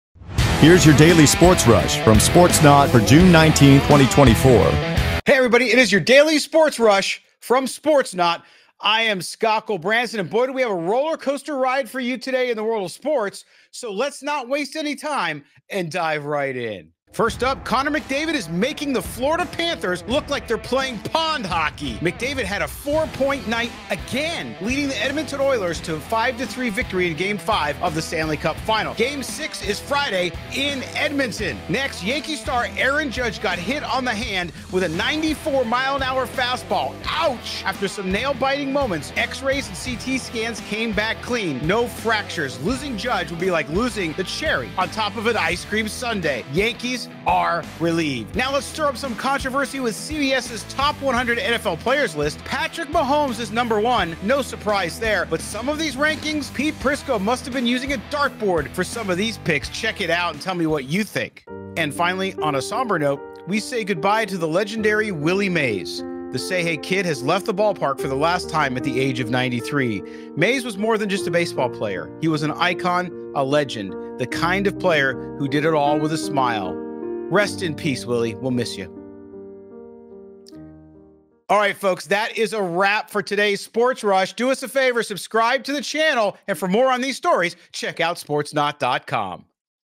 Daily Sports Rush: McDavid Shines, Judge Escapes Injury, and Mahomes Tops NFL List!
Welcome to your Daily Sports Rush from Sportsnaut! Hosted by Scott Gulbransen, today we cover Connor McDavid's stellar performance for the Edmonton Oilers in the Stanley Cup Final, Aaron Judge's injury scare for the Yankees, CBS's controversial NFL Top 100 list, and a tribute to the legendary Willie Mays. Catch all the latest sports updates right here.